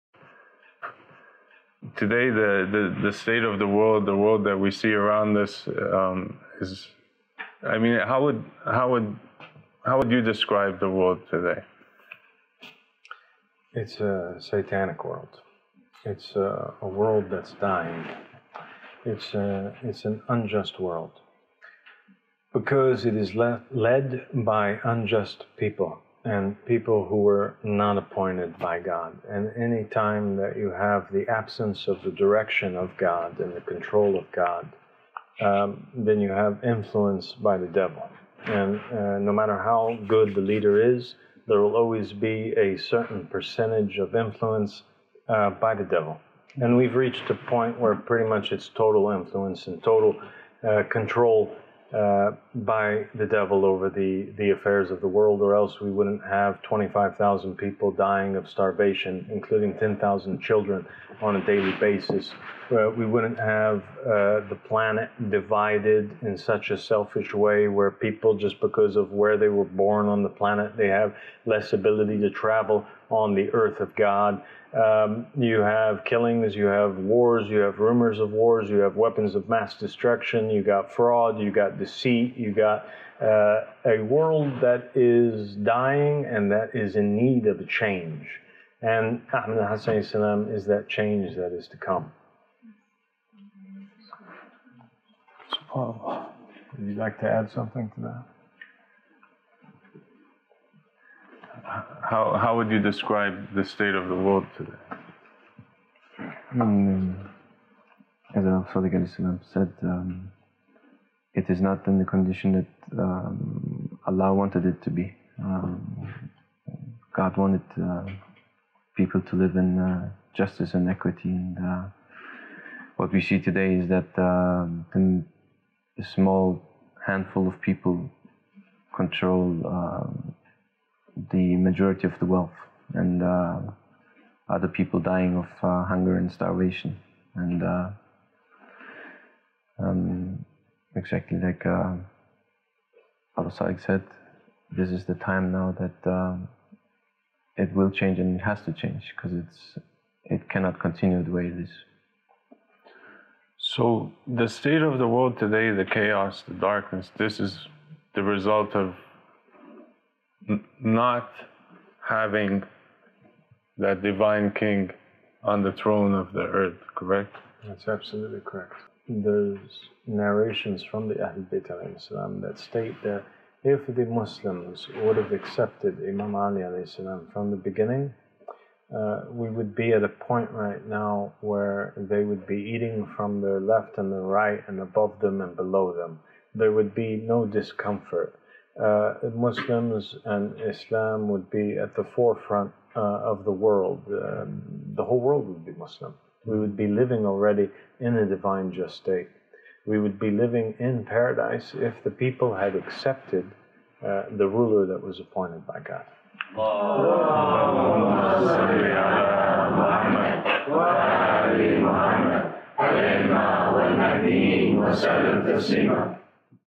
¿Cómo ve el mundo actual?, How do you see the world today?
¿Cómo describiría el estado del mundo actual? Esa fue la pregunta que se planteó en esta parte de la conversación con el Siervo de Dios.

How would you describe the state of the world today? That was the question posed in this part of the conversation with the Servant of God.

كيف تصف وضع العالم اليوم؟
كان هذا هو السؤال الذي طُرح في هذا الجزء من الحوار مع عبد الله. للمزيد من المعلومات
قوموا بزيارة